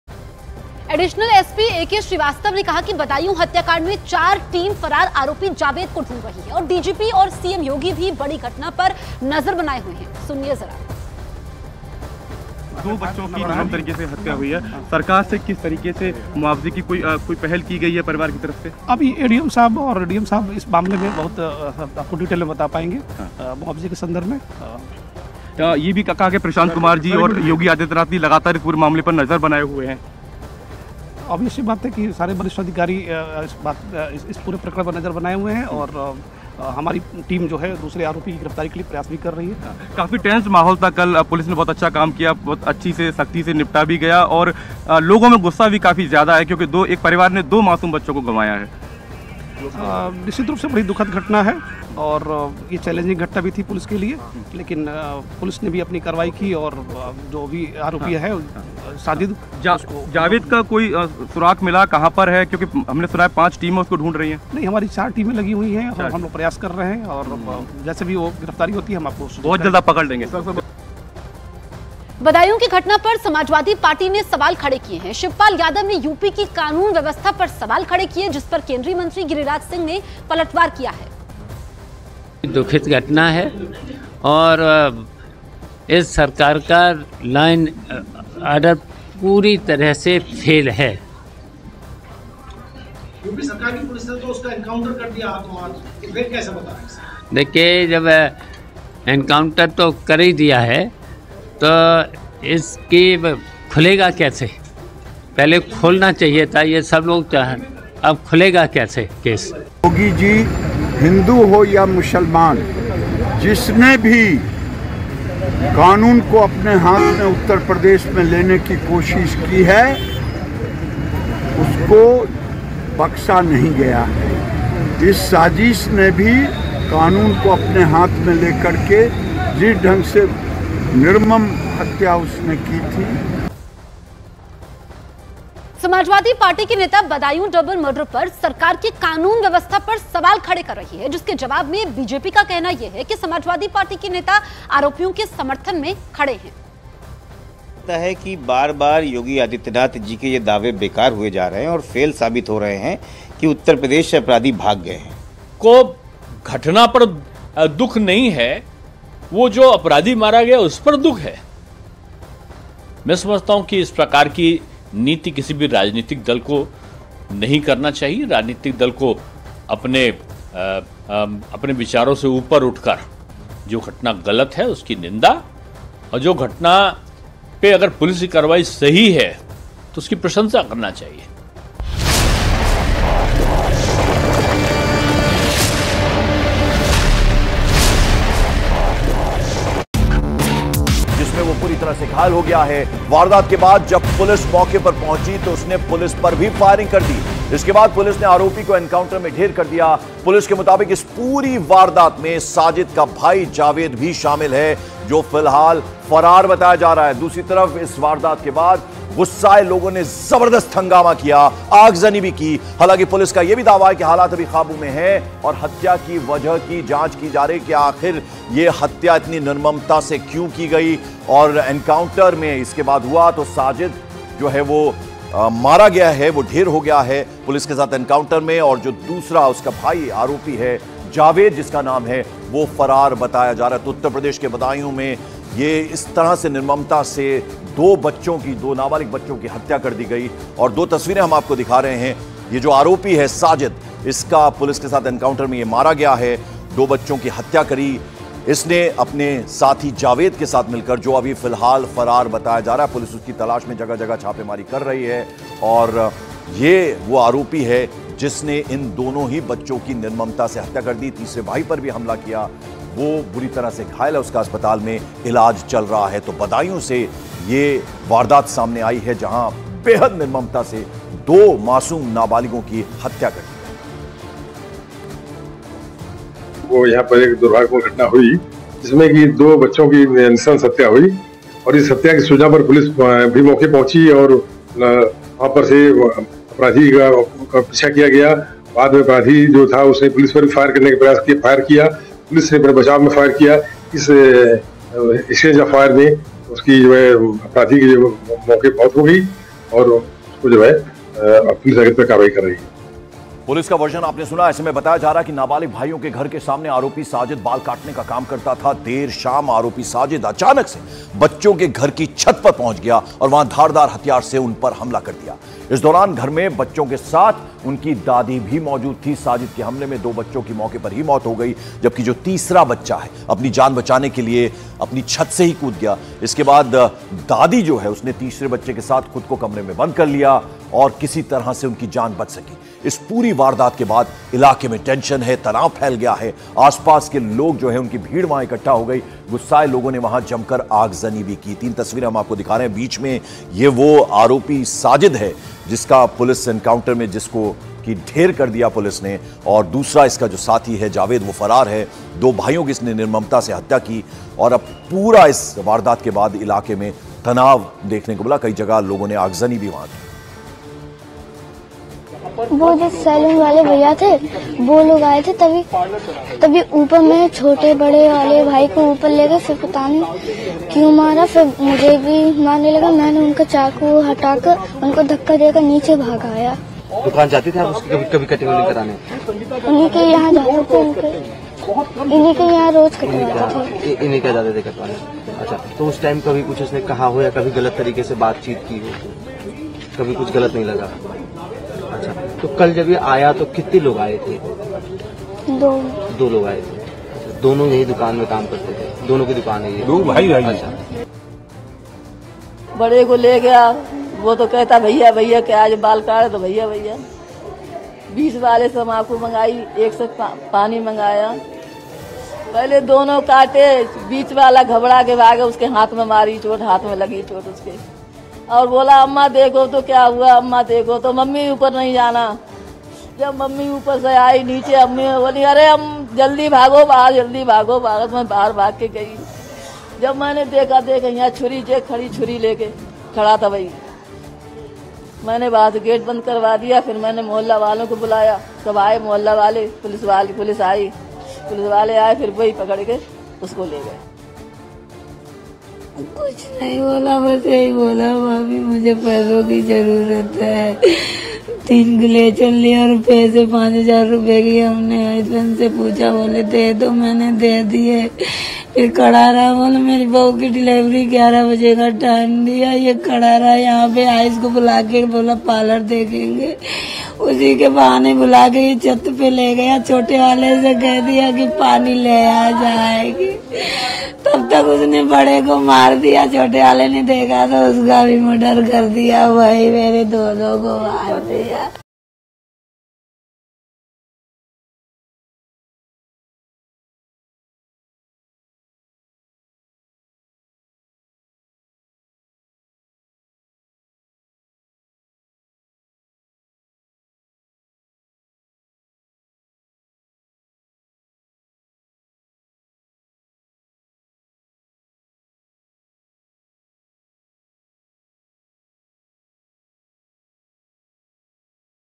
UP News : Budaun में ख़ौफ़नाक क़त्ल, घटना के कुछ घंटे बाद आरोपी का एनकाउंटर | Uttar Pradesh | Police
Up News: Additional SP A.K. Srivastava said that 4 teams are searching for absconding accused Javed in the Badaun murder case. And DGP and CM Yogi are also keeping an eye on the big incident.

Up News : एडिशनल एसपी ए.के. श्रीवास्तव ने कहा कि बदायूं हत्याकांड में 4 टीम फरार आरोपी जावेद को ढूंढ रही हैं। और डीजीपी और सीएम योगी भी बड़ी घटना पर नजर बनाए हुए हैं।